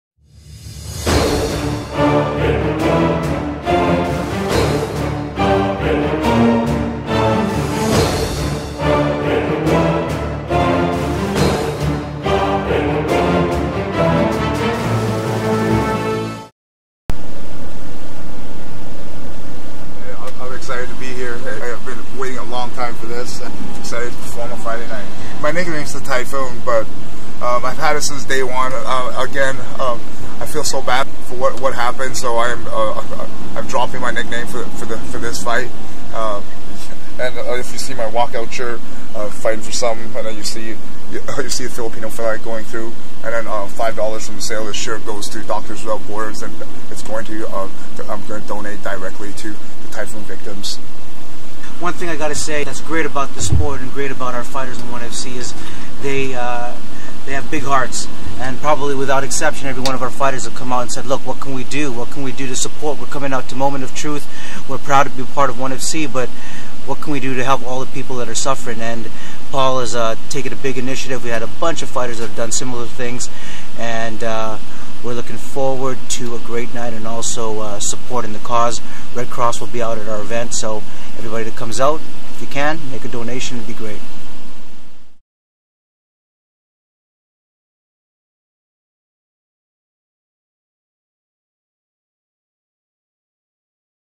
Paul Cheng drops his Typhoon nickname for ONE FC: Moment of Truth in respect to Yolanda victims
Paul Cheng has always been known as The Typhoon but he will drop this for his fight against Alain Ngalani out of respect to the horrible typhoon that devastated the Philippines.